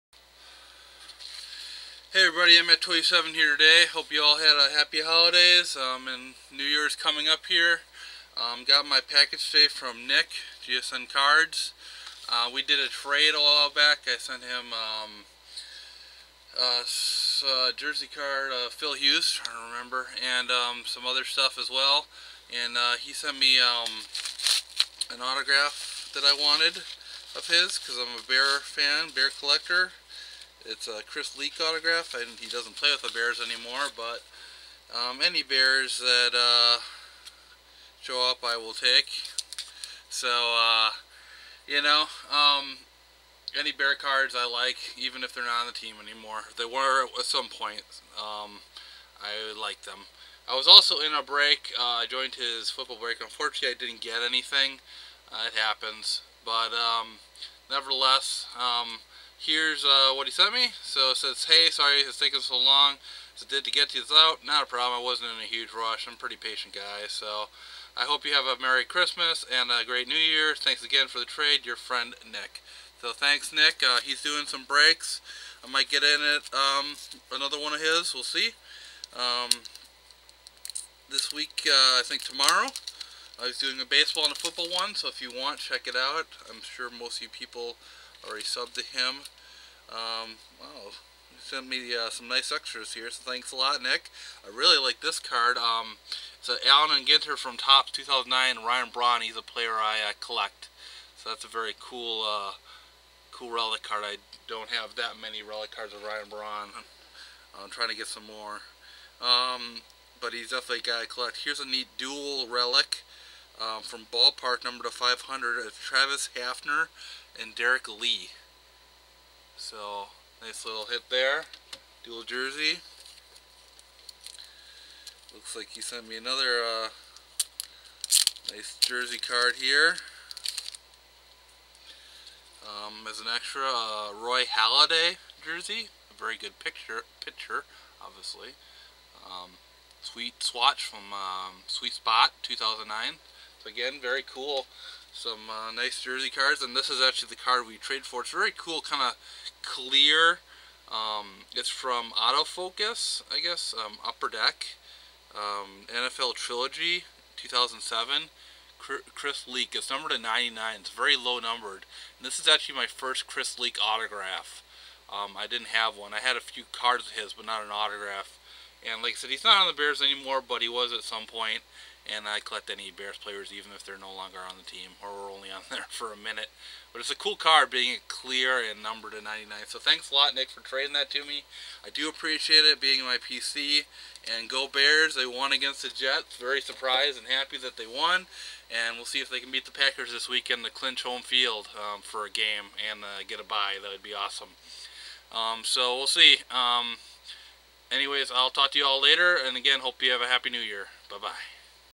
1 Package Mail Day from GSNCards, Bears Auto & extra's from a trade
Got a package in from a trade I did with Nick aka GSNCards.  Here's what was in it.